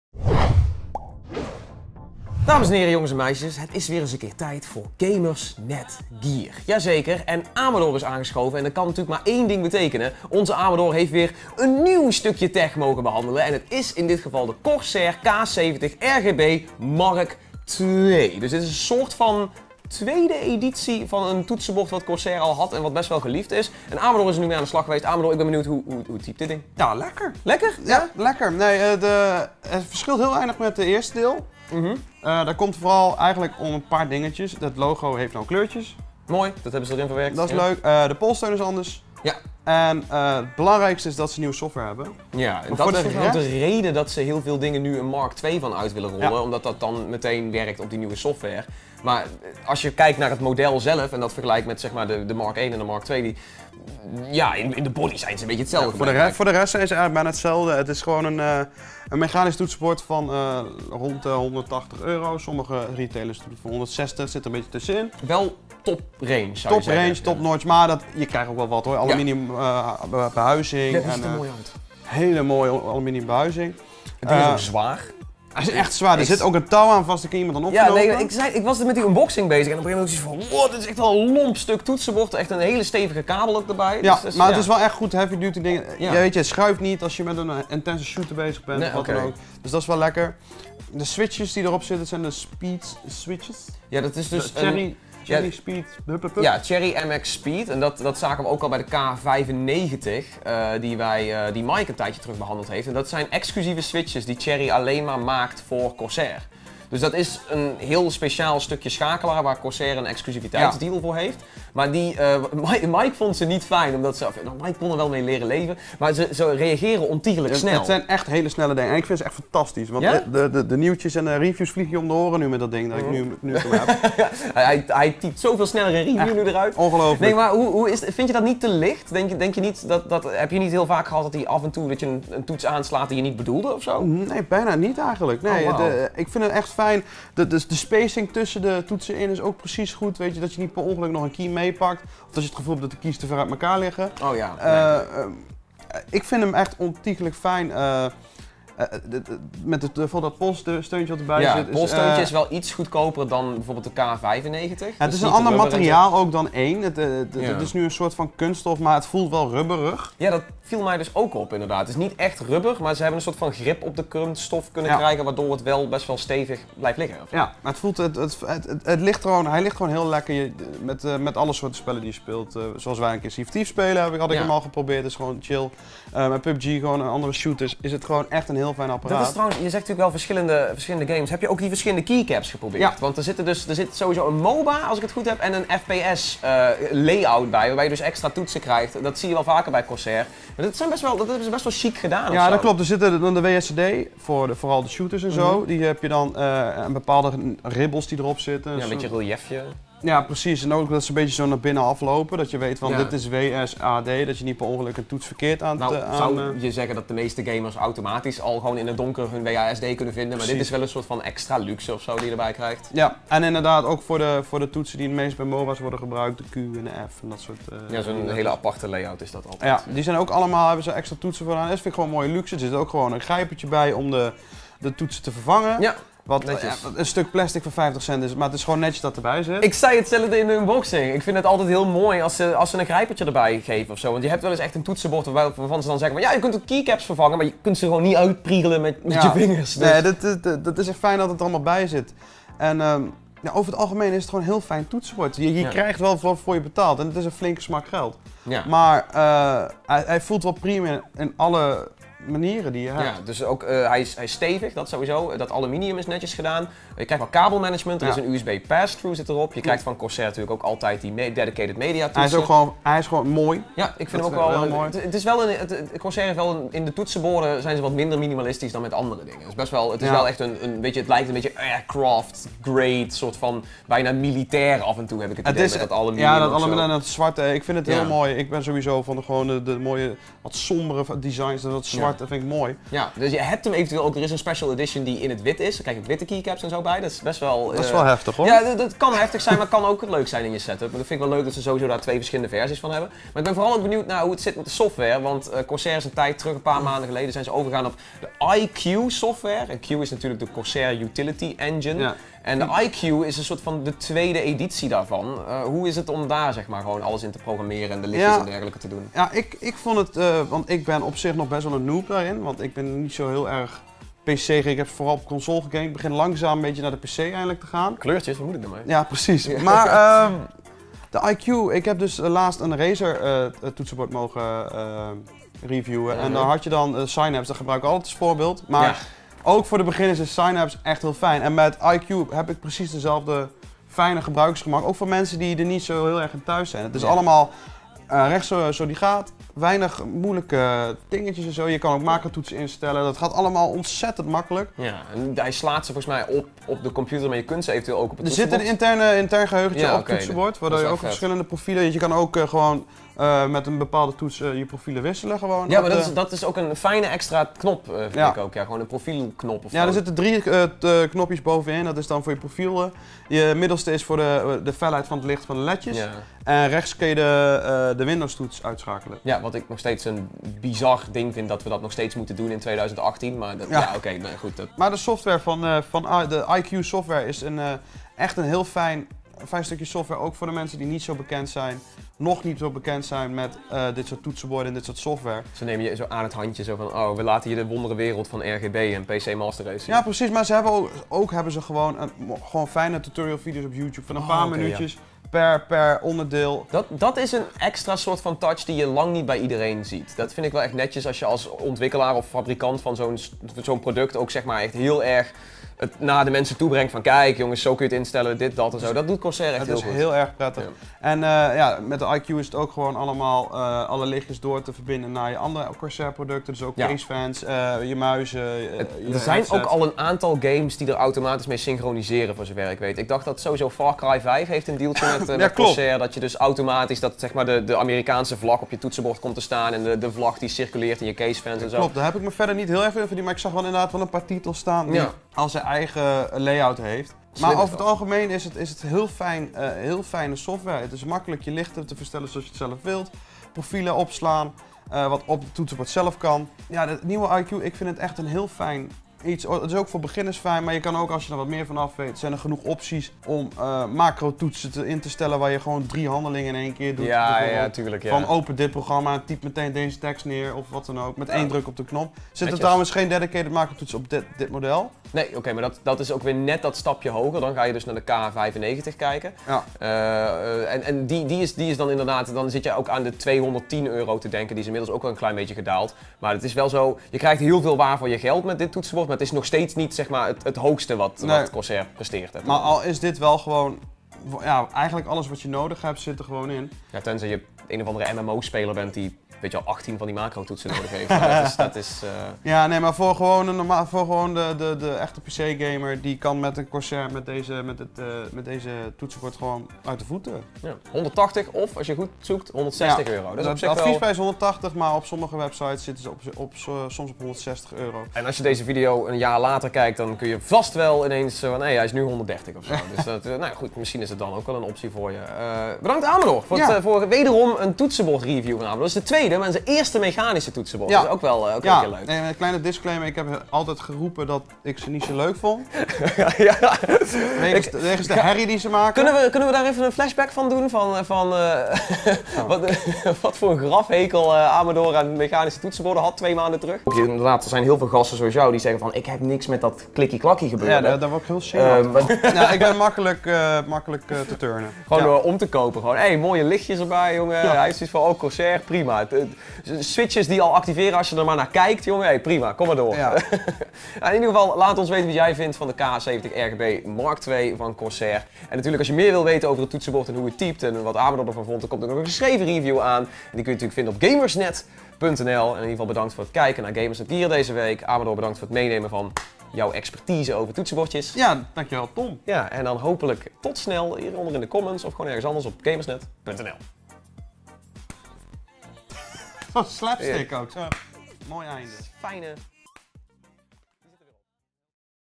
Hoe Corsair de K70 vernieuwt met MK.2 | Gamersnet Gear
Al vluchtig te zien in onze unboxing ), maar nu ook volledig door de review van onze Amador!

Hoeveel verbetert Corsair het iconische K70-toetsenbord met deze MK.2-variant? Je hoort en ziet het hier, in wederom een nieuwe aflevering van Gamersnet Gear...